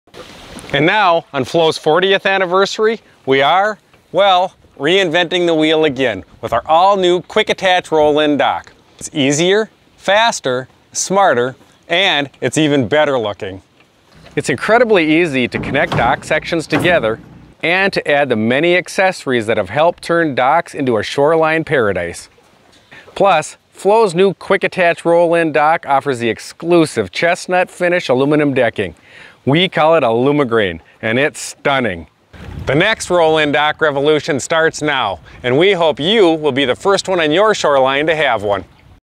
Introducing the new Quick-Attach roll-in dock.
Introducing the new Quick-Attach roll-in dock. It’s Easier, Faster, Smarter, and even better looking. The next roll-in dock revolution starts now!